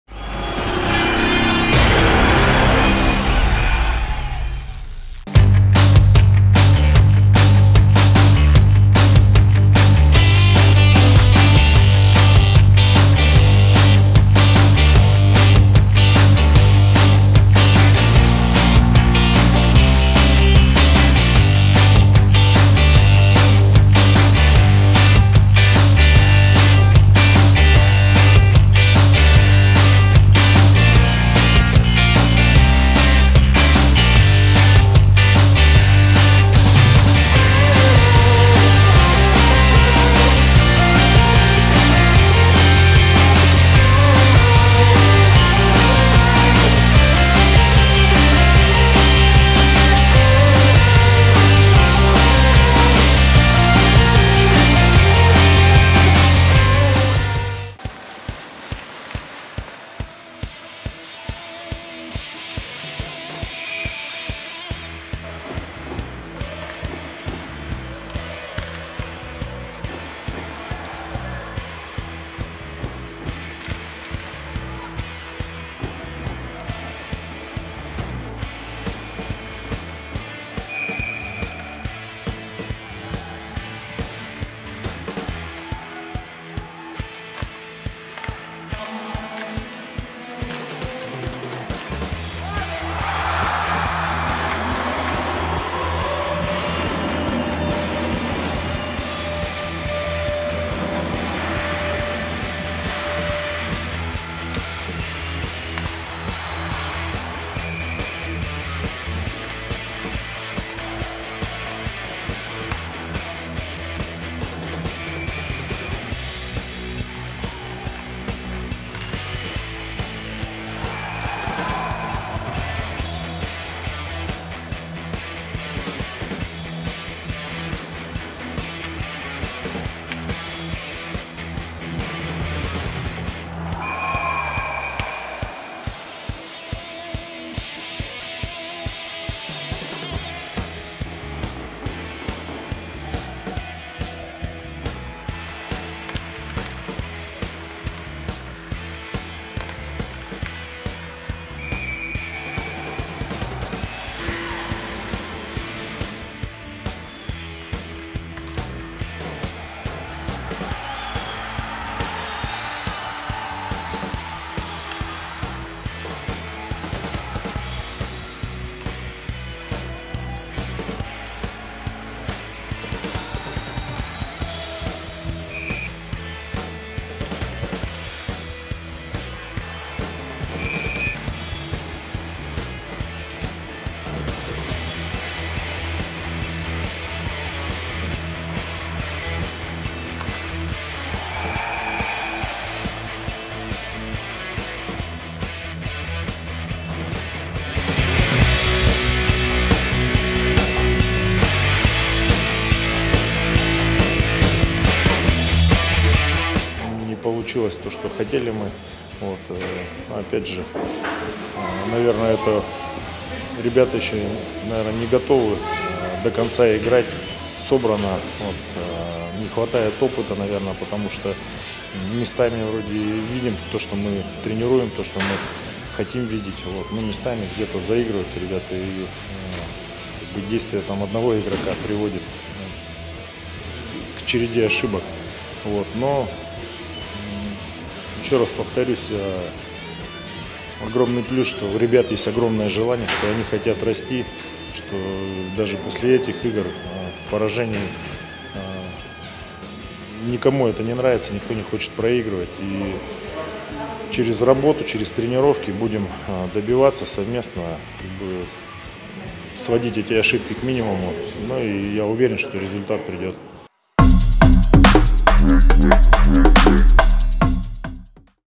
Белые Медведи vs Ирбис - 1:3. 26 Сентября 2015
Все голы и лучшие моменты матча «Белых Медведей» против «Ирбиса» и комментарий Анвара Гатиятулина.